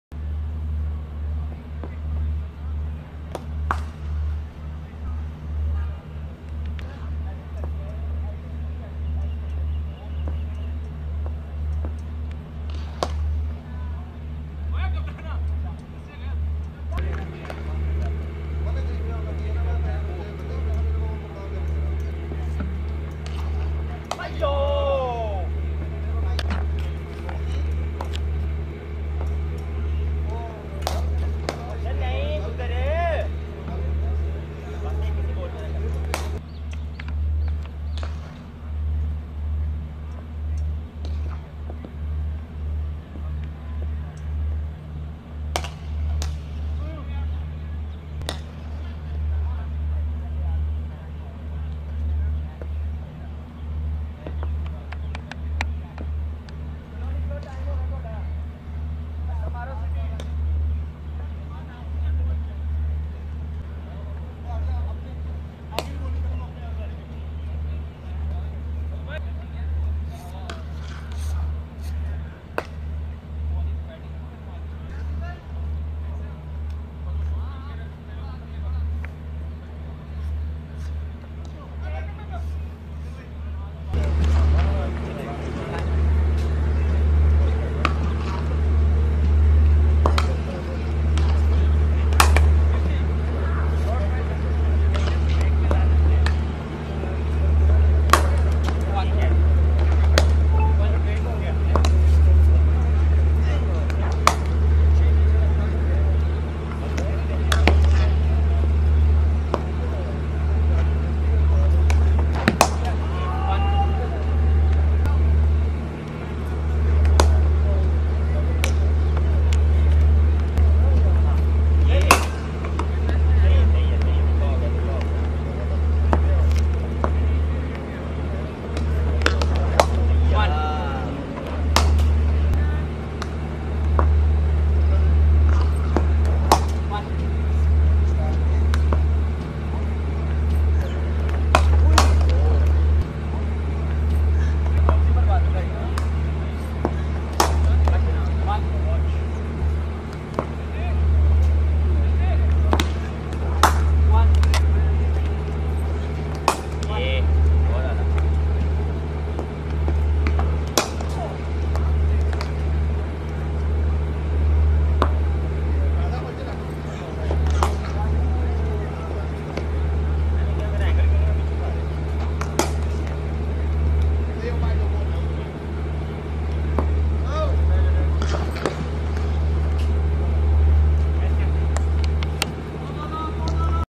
Babar Azam First time bowling at practice session at pindi cricket stadium, Rawalpindi today
Babar Azam bowling in practice session Today